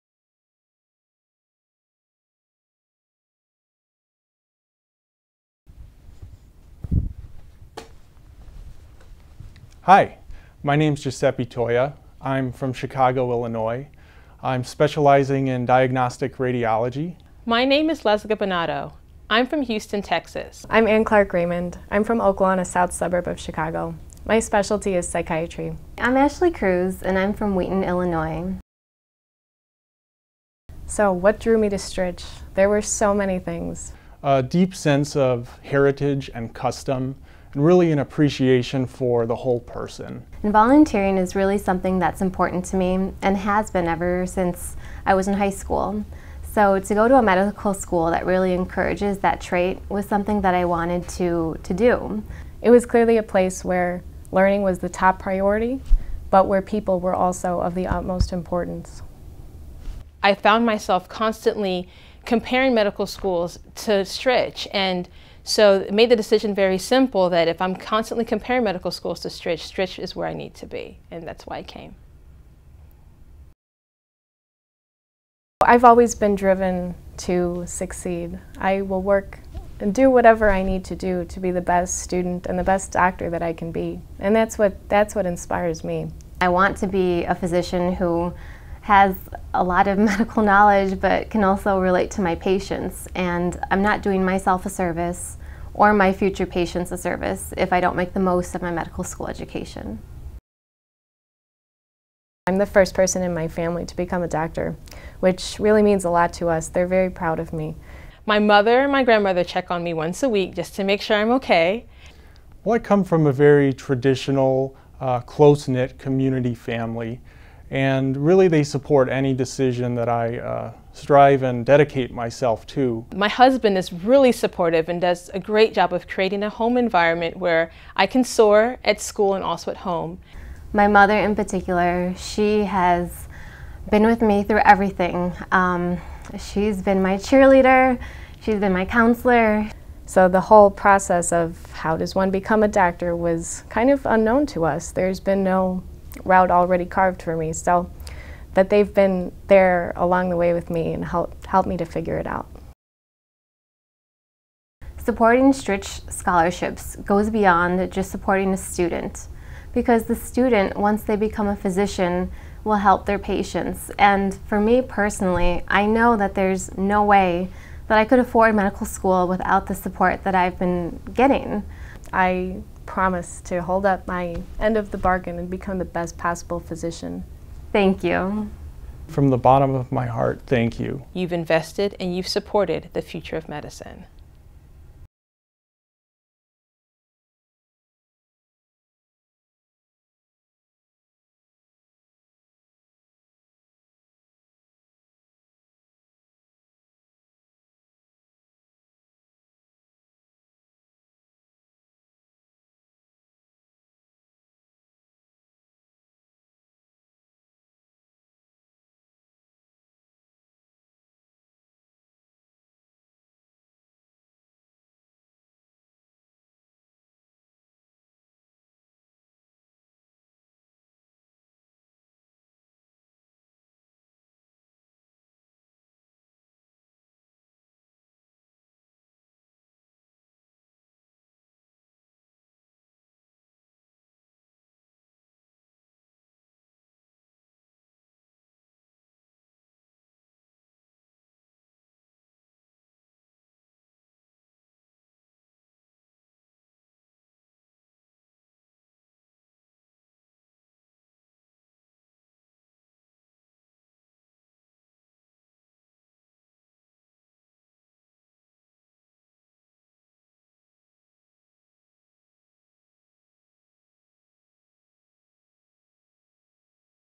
Stritch School of Medicine Scholarships Reunion Weekend
Loyola University's Stritch School of Medicine is a prestigious medical school that teaches students to treat the whole person. Faculty and staff are dedicated to teaching the students how to prioritize and relate to patients.  To learn more about how you can invest and support the future of medicine, visit LUC.edu/Stritch/giving